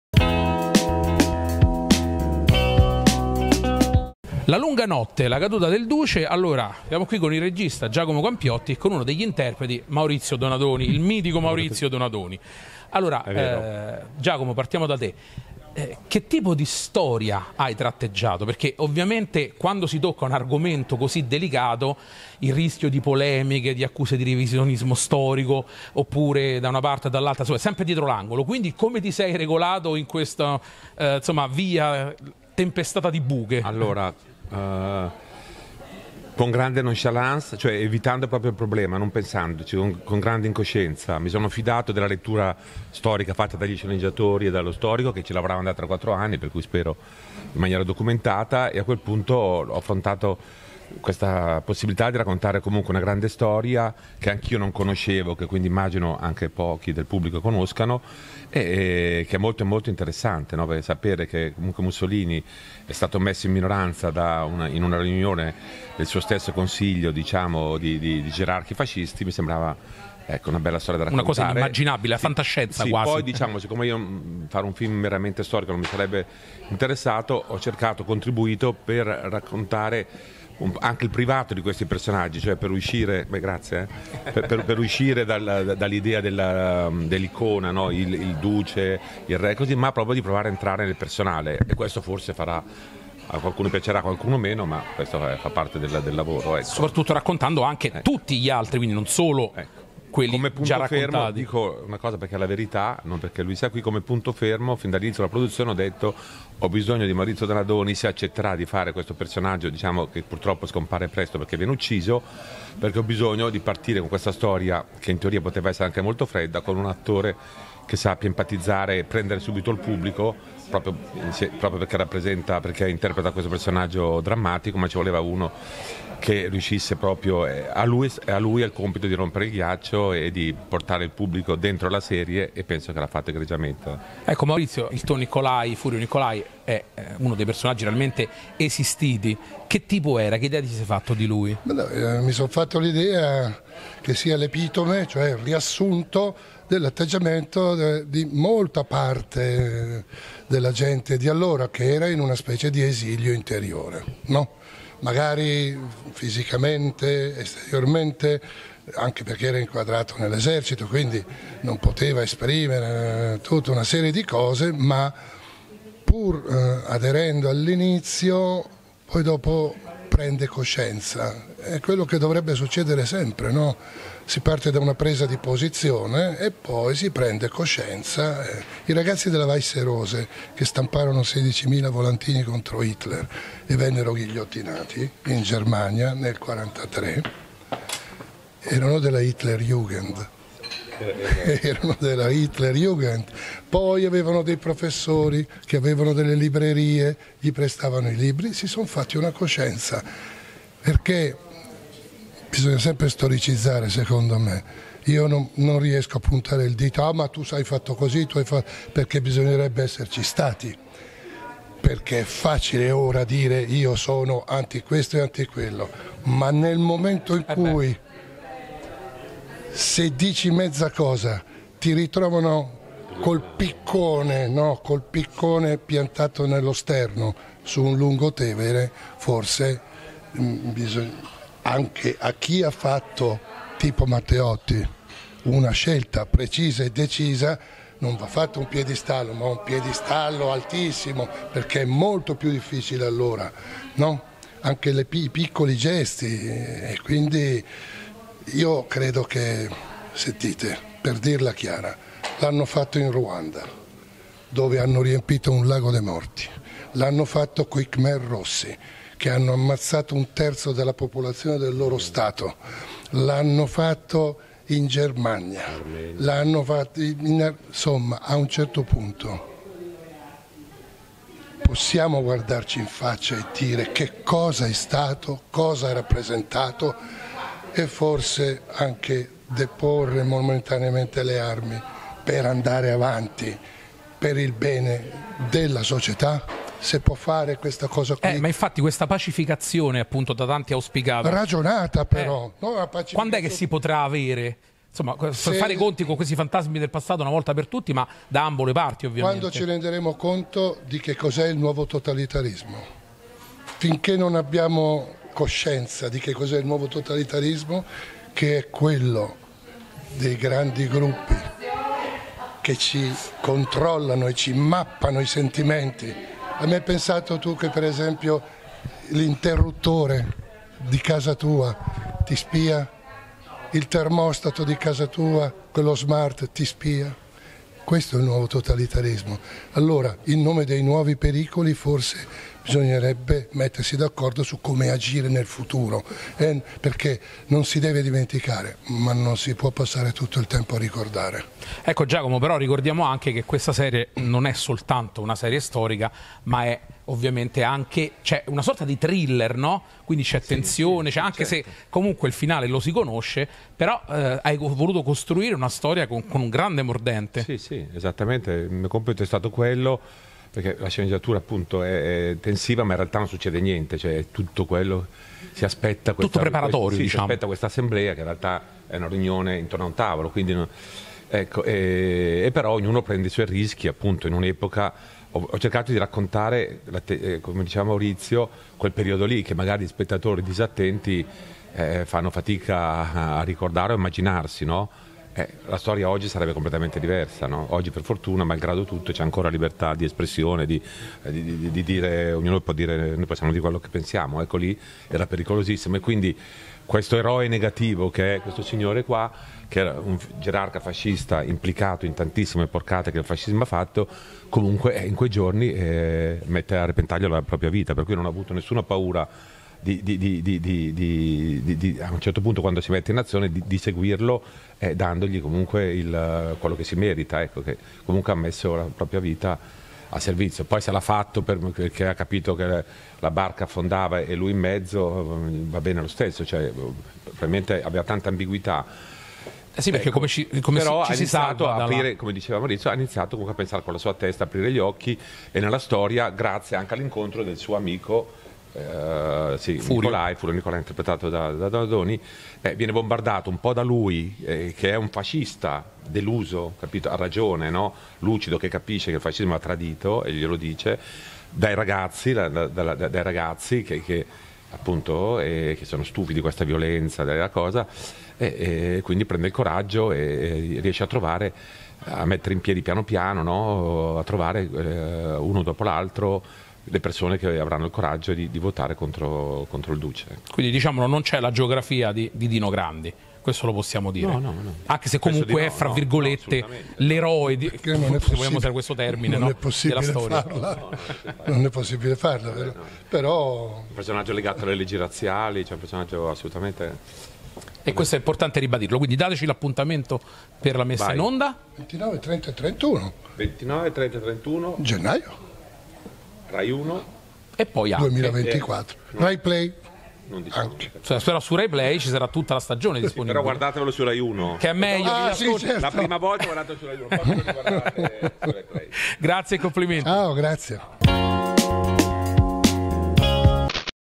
Giacomo Campiotti e Maurizio Donadoni, intervista La lunga notte - La caduta del Duce
Vi presentiamo la nostra video intervista al regista Giacomo Campiotti e all'attore Maurizio Donadoni (Furio Nicolai) per la miniserie "La lunga notte - La caduta del Duce".

Video intervista di Ivan Zingariello.

Guarda le nostre video interviste al cast:
Alessio Boni

Sinossi: Sceneggiata da Franco Bernini e Bernardo Pellegrini, con la consulenza storica di Pasquale Chessa, La Lunga Notte narra le tre settimane precedenti la notte tra il 24 e il 25 luglio 1943 in cui si svolse l’ultima riunione del Gran Consiglio, organo supremo presieduto da Benito Mussolini, che segnò la fine del regime fascista.

La serie ripercorre i fatti che condussero a quel momento cruciale, raccontando la Storia con la S maiuscola insieme alle storie di uomini e donne che agirono da protagonisti e misero in gioco il loro destino personale oltre a quello del Paese.

Le splendide location della Capitale si alternano a luoghi altrettanto evocativi nell’hinterland romano e si fondono con i costumi e auto d’epoca per richiamare con grande potenza narrativa gli affanni di Dino Grandi, Presidente della Camera dei fasci, che decise di opporsi alle scelte di Mussolini in maniera legittima convocando il Gran Consiglio per rimuovere il Paese dalle mani del Duce. Accanto alle sue vicende, quelle della famiglia Reale, di Edda e Galeazzo Ciano, di Claretta Petacci, dei vari gerarchi e di tanti altri combattuti dalla paura e dall’ambizione al potere.